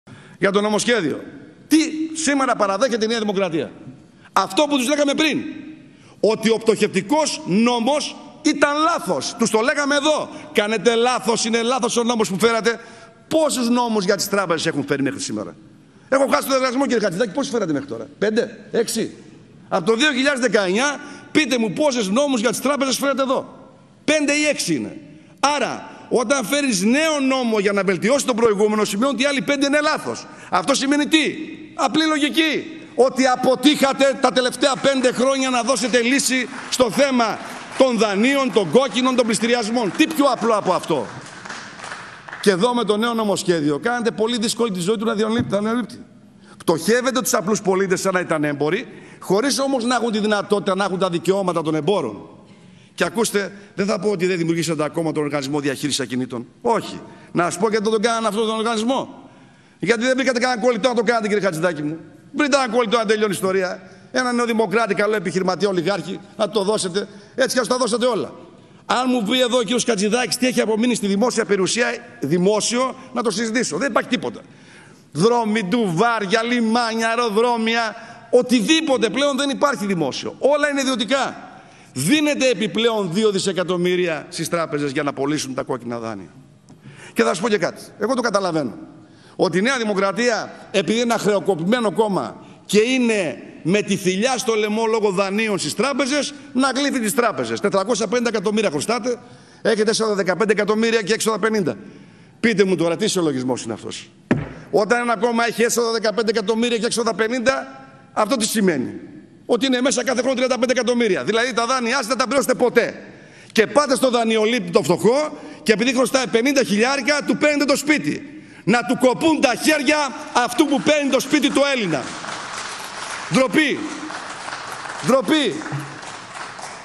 Να του κοπούν τα χέρια αυτού που παίρνει το σπίτι του Έλληνα!
Κεραυνοί  Βελόπουλου για τη ΝΔ  και τις τράπεζες! || ΒΟΥΛΗ 01/12/2023